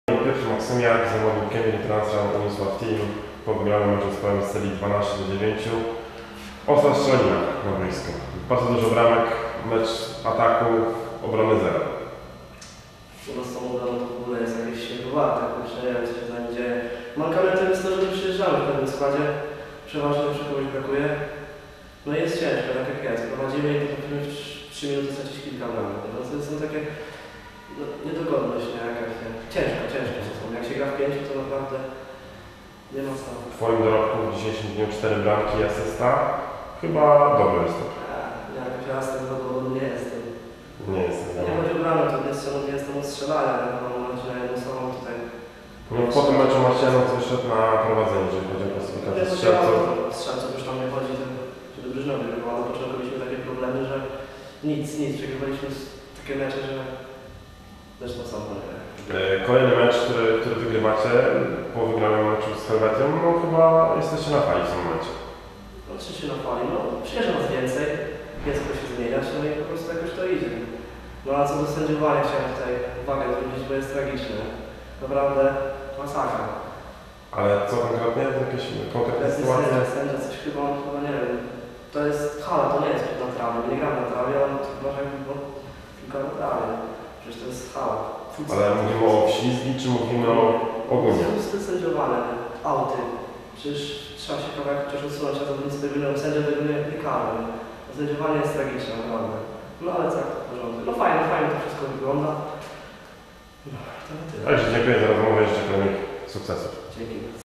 Wywiad z Piotrem Maksymiakiem - Transrem/Kemira Unisław Team po meczu z Sylą 12 - 9 (16-01-2011)
IV Edycja HLPN "Stalex Liga w Świeciu. Wywiad z Piotrem Maksymiakiem  - Transrem/Kemira Unisław Team po meczu z Sylą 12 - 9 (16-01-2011)